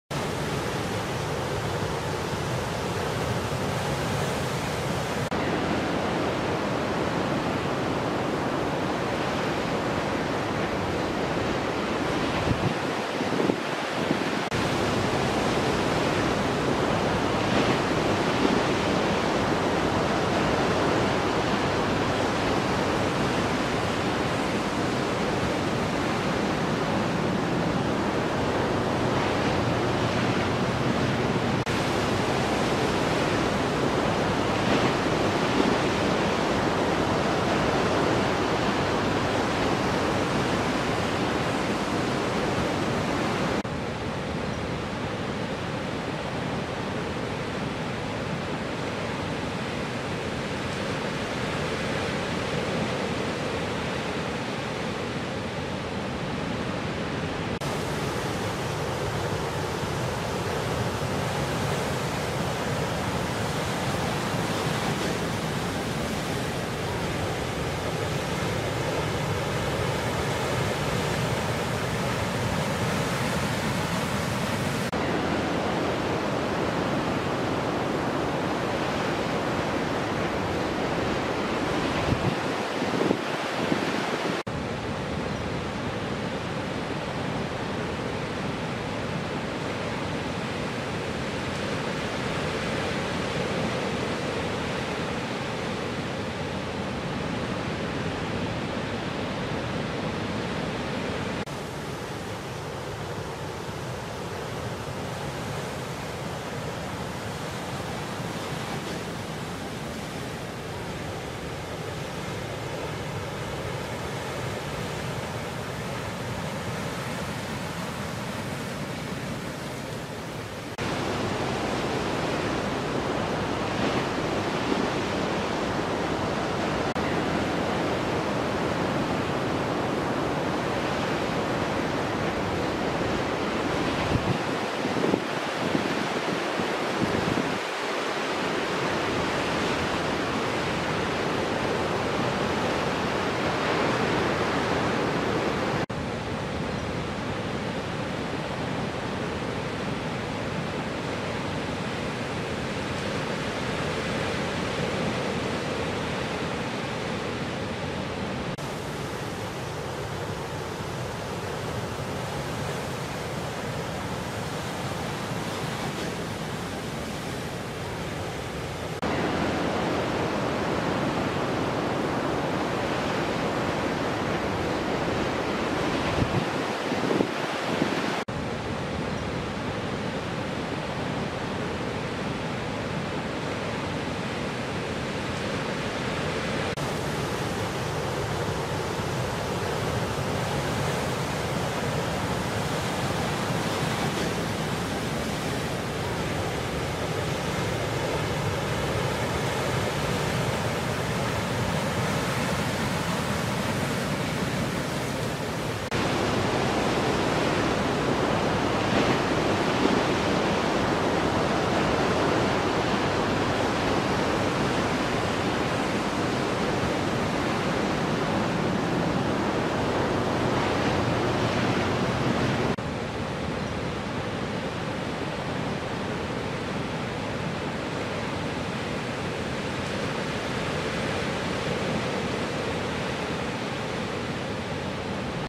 Bad Timing as Snapper Fires up.
Everyone who regularly surfs behind the rock at Snapper has experienced the backwash and how one minute you think you are dropping into a bomb then suddenly a wobble then teh wave just explodes sending you skywards.